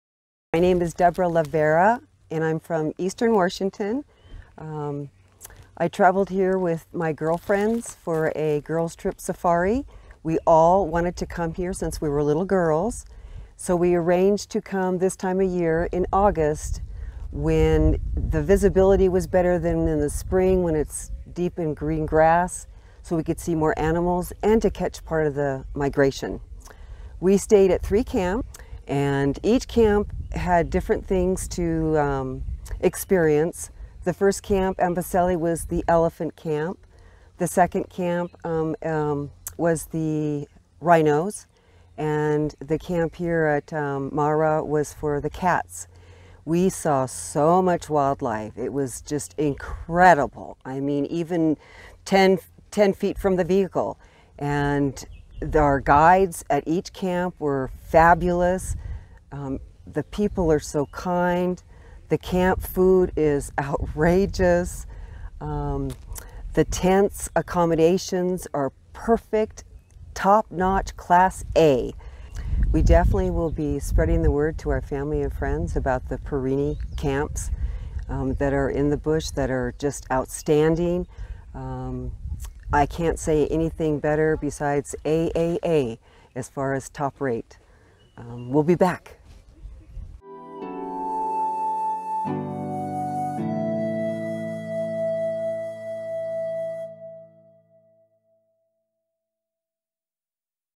The Tents, Accommodation Were Perfect ,Top-notch, Class A - Porini Guest from USA in Maasai Mara
Debra Levaara came to Kenya for a girls' trip with her friends from the USA and stayed with us at Porini Amboseli, Porini Rhino and Porini Mara Camps. She was amazed that each camp had its own unique wildlife to offer. Porini Amboseli Camp is located in Selenkay Conservancy in Amboseli, Porini Rhino Camp is in Ol Pejeta Conservancy, Nanyuki while Porini Mara Camp is based in Ol Kinyei Conservancy, Maasai Mara.